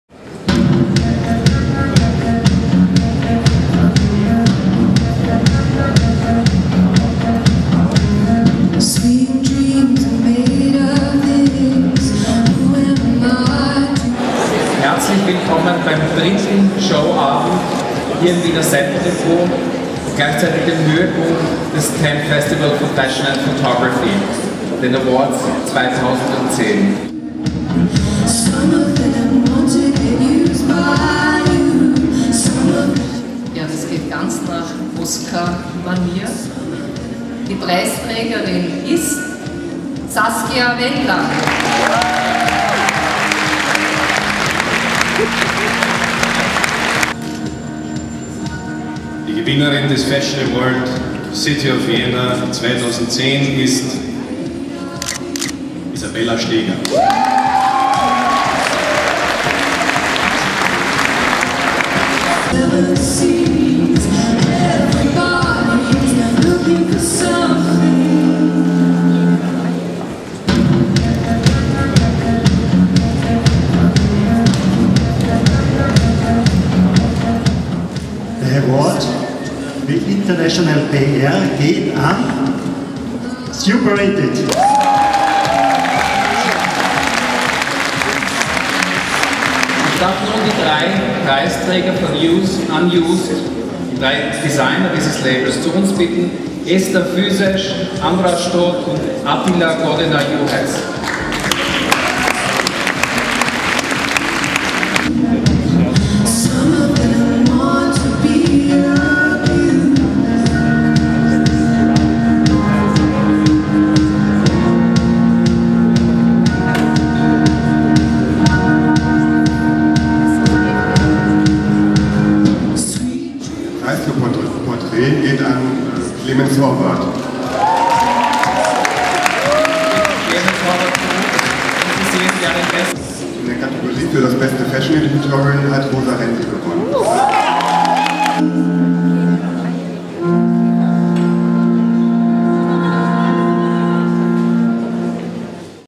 Austria Fashion Awards AFA 2010
Preisverleihung der Austria Fashion Awards AFA 2010

Die AFA 2010 Gewinner:

Modepreis des BMUKK
Saskia Wendland

Modepreis der Stadt Wien
Isabelle Steger

Unit F Award for International PR
superated

KONTAKT. Fashion Award by Erste Bank
USE unused

Unit F Editorial Award for Fashion Photography
Klemens Horvath (Category Portrait)
Rosa Rendl (Category Fashion)

Moderator / Preisverleiher
Andreas Oberkanins, CEO Unit F
Claudia Schmied, Bundesministerin
Andreas Mailath-Pokorny, Stadtrat
Paulus Stuller, WKW Vizepräsident

Video Visuals
by Mark Glassner